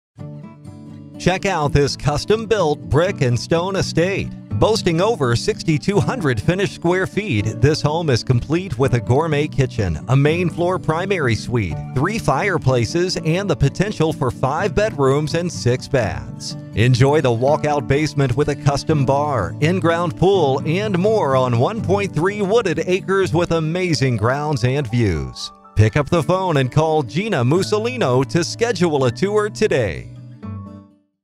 622 Versailles Drive Valparaiso, IN | ColdwellBankerHomes.com
622 Versailles Drive, Valparaiso, IN

Custom built, all brick and stone estate, in gated St. Andre, boasting over 6,200 finished square feet with walk-out basement, custom bar, five bedroom potential, three fireplaces, six baths, a gourmet kitchen, main-level primary bedroom suite with a private bath, attached four-car garage, secluded